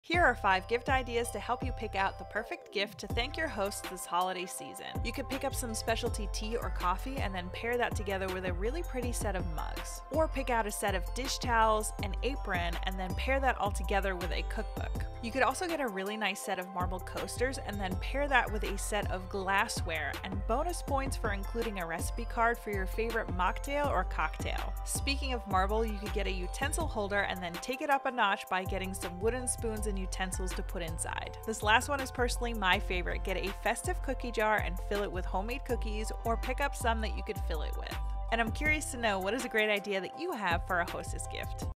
Holiday Host Gift Ideas
Do you need some Holiday Host Gift Ideas for surprising your host this season? We hope these gift ideas spark some inspiration for you this holiday.

🎥 MAKING YOUR OWN VIDEOS ON YOUTUBE?🚀
Tools and Services We Recommend: